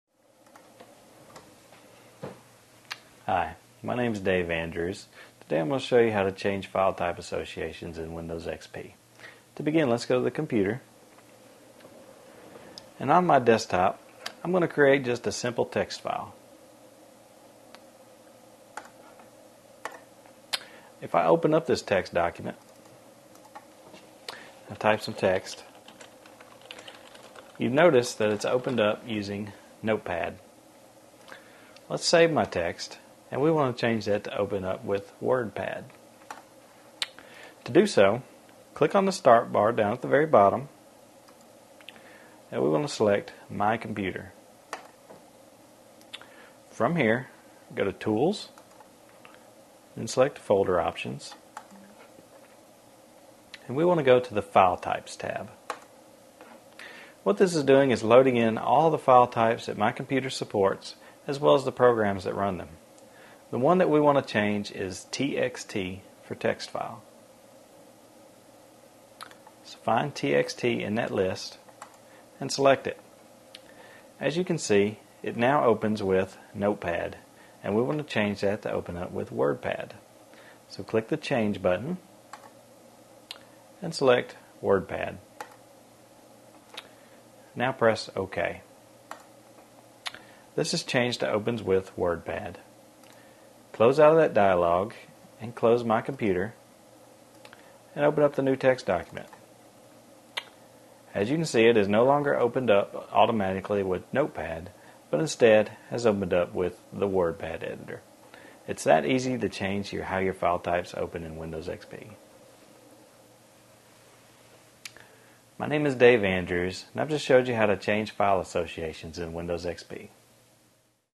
How to Change File Type Associations in Windows XP
How to Change File Type Associations in Windows XP. Part of the series: Computer Software & Hardware Advice. Change computer file type association in Windows XP by opening up a text document, clicking Start, My Computer, Tools, Folder Options and going to the File Types tab. Change Windows XP file type associations with tips from a software developer in this free video about computer technology.